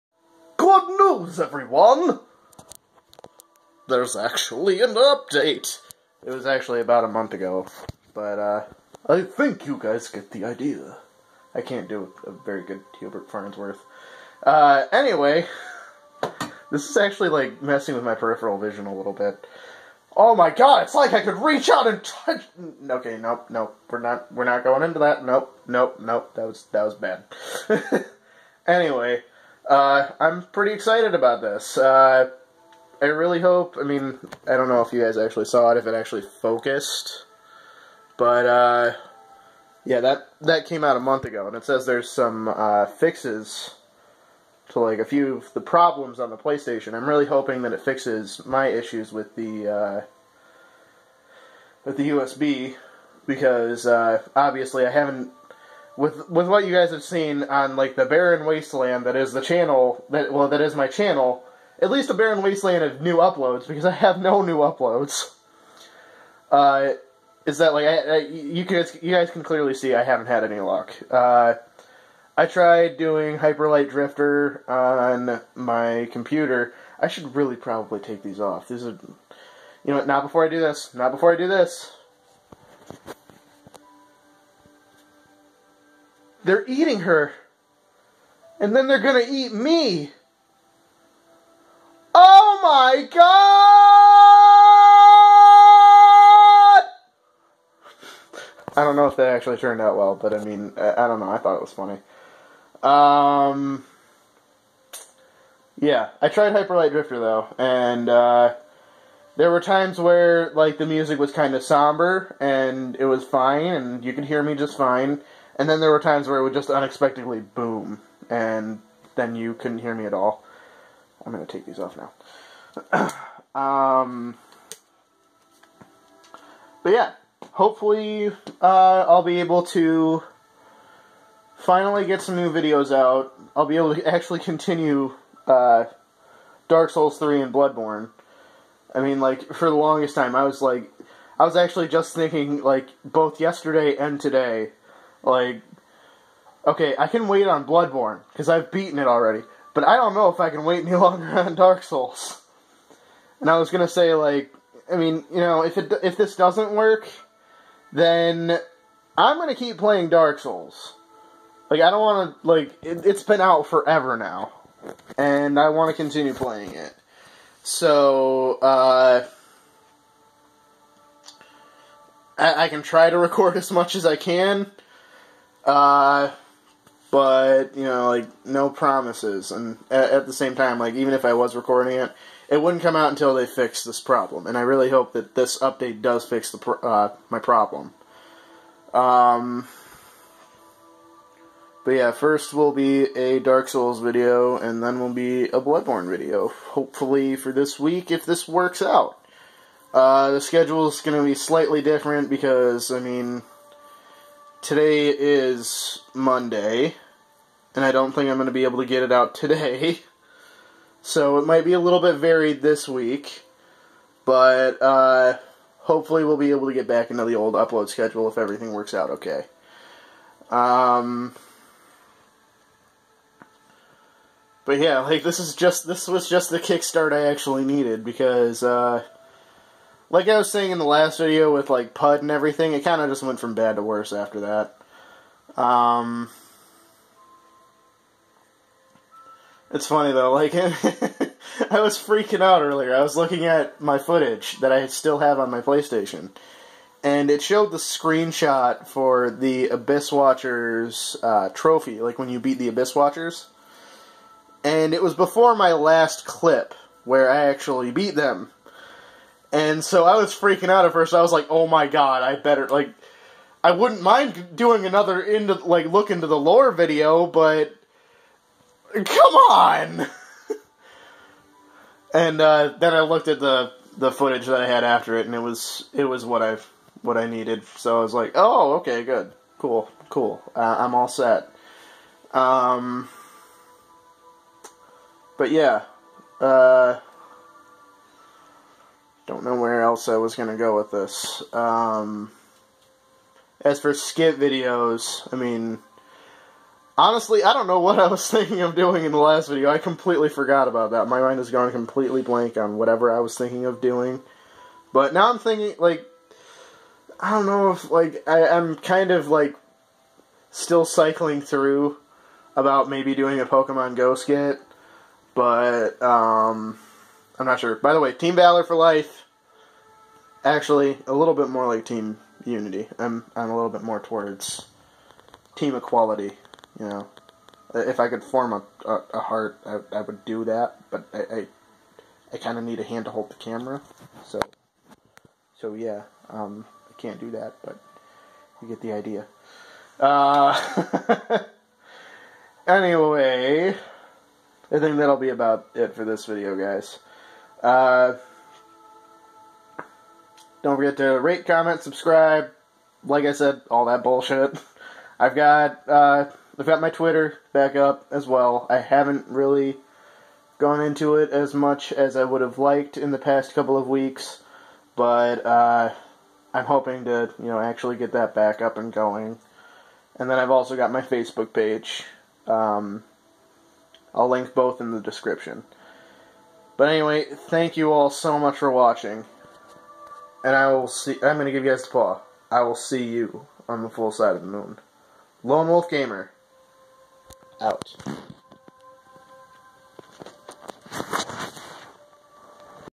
Lonewolf update #33 Hope also Pokemon Gooooo!!!
A new update rolled out for PlayStation 4 about a month ago I'm excited to say I'm downloading it now and if all goes well new videos will be coming this week